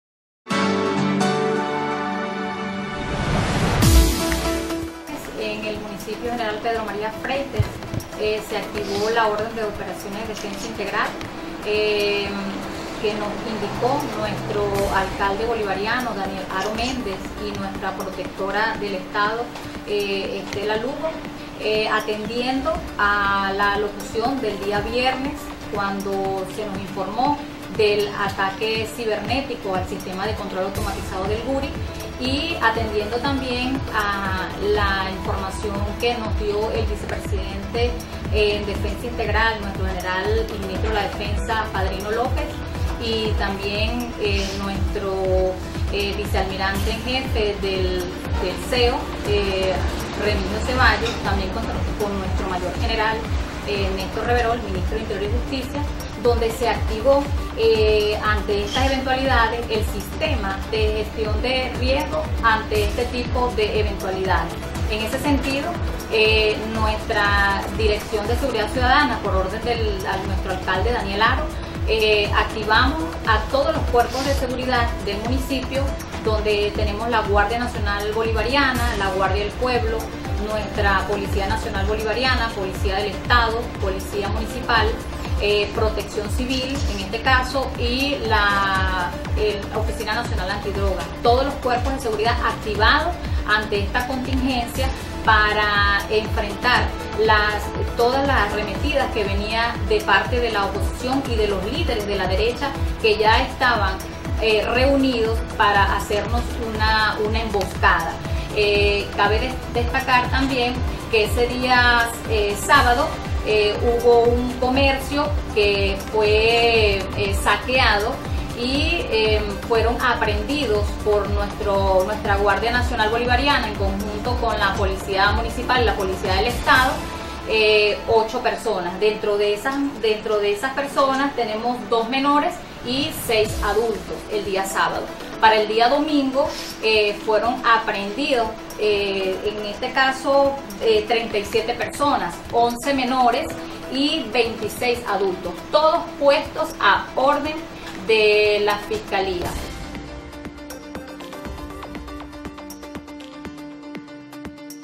Directora de Seguridad Ciudadana Alcaldía del Mcpio Freites, Mabel Bravo
La Directora de Seguridad Ciudadana de la Alcaldía del Municipio Bolivariano Gral Pedro María Freites, Mabel Bravo, ofrece balance sobre la activación de la ODDI